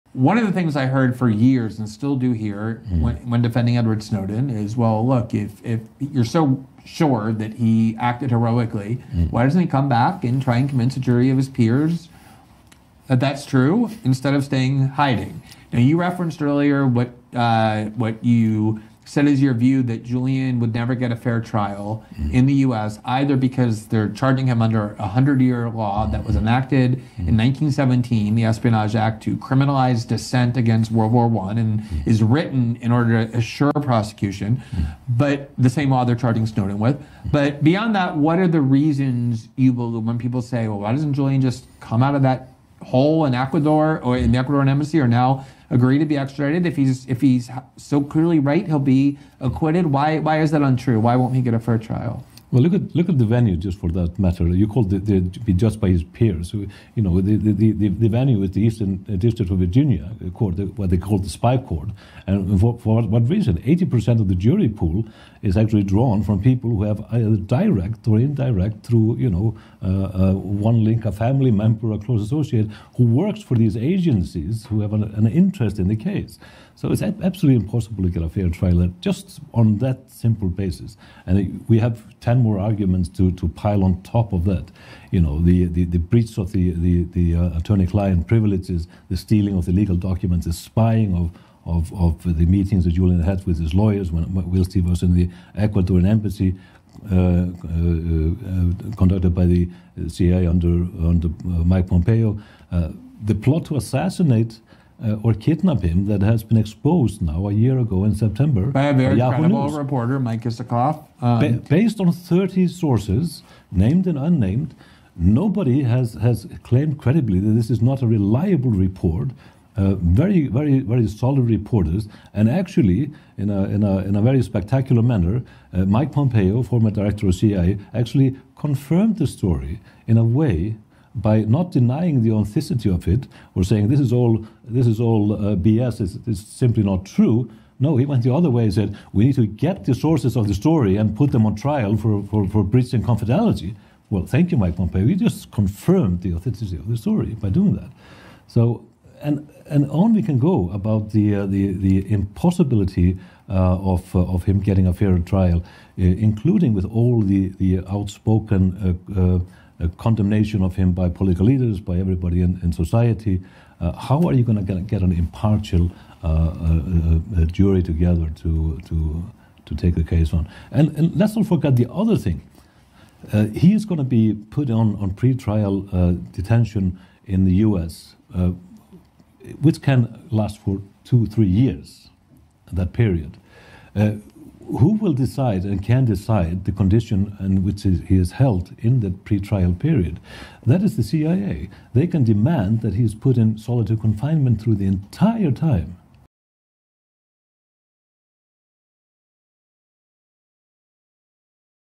No Fair Trial In The US
Multiple claims that the United States broke the law in relation to the Assange case such as spying on him and his associates in the Ecuadorian embassy or the CIA plot to have him killed have never been truly denied. It would be impossible for him to receive a fair trial in the US